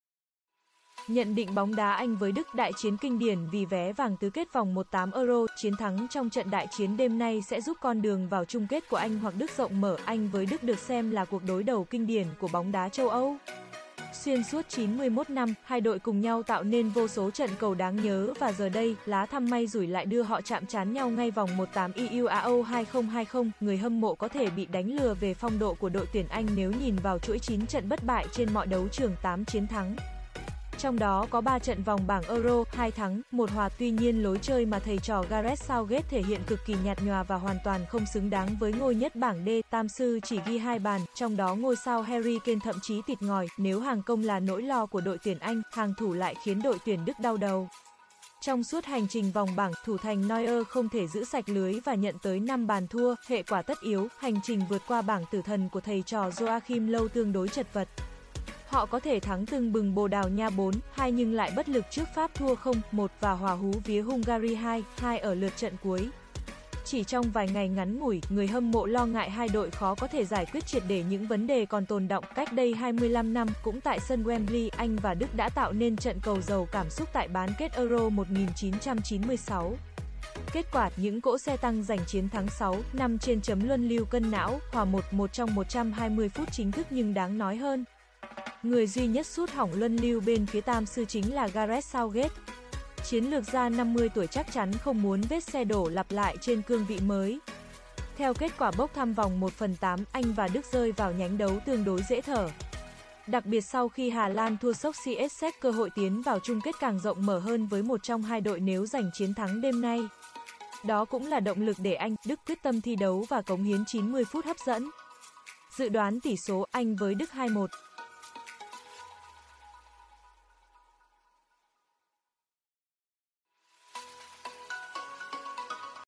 Soi kèo Anh vs Đức 23h00 ngày 29/6/2021| Nhận định Dự đoán kết quả Vòng 1/8 Euro 2020
Tuyển Anh gặp tuyển Đức tại sân Wembley vào đêm nay trong cuộc đọ sức hứa hẹn là một trong những trận đấu đáng chú ý nhất của VCK EURO 2020.
Chương mới nhất của một trong những cuộc đại chiến của bóng đá quốc tế sẽ diễn ra trên sân Wembley - giống như cách đây 25 năm khi cú sút không thành công trên chấm 11 m của Southgate đã giúp Đức vượt qua Anh trong loạt đấu súng cân não tại EURO 96. Sự chặt chẽ có thể xảy ra một lần nữa khi “Tam sư” chỉ ghi được 2 bàn thắng ở vòng bảng trong lúc “Mannschaft” chỉ tạo ra được một màn trình diễn mãn nhãn trước Bồ Đào Nha.
Đội hình dự kiến
Anh (4-2-3-1): Pickford - Walker, Stones, Maguire, Shaw - Rice, Phillips - Foden, Mount, Sterling - Kane.
Đức (3-4-3): Neuer - Ginter, Hummels, Rudiger - Kimmich, Goretzka, Kroos, Gosens - Havertz, Mueller, Gnabry.